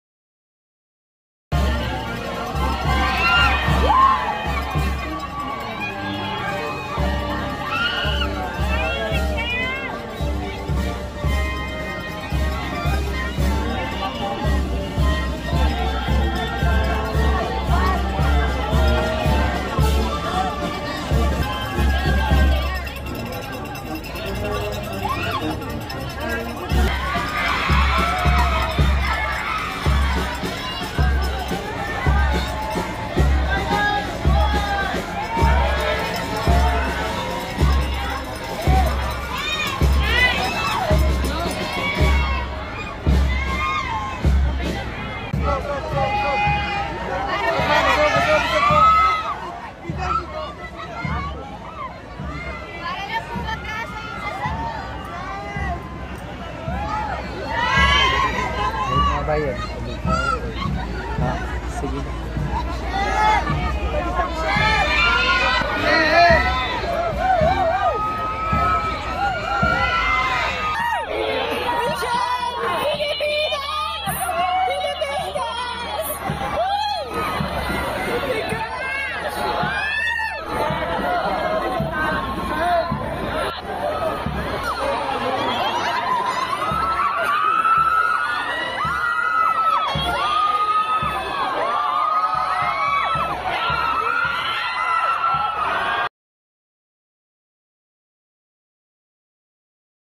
MICHELLE DEE HOME COMING PARADE in MALL OF ASIA | Braver Gey
THE true queen of miss Universe 2023